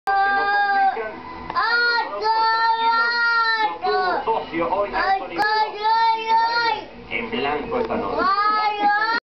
CABALLO toby
mi sobrino toby viendo caballos y a él le encantan. y dice caaalloooo mi huasito mas lindo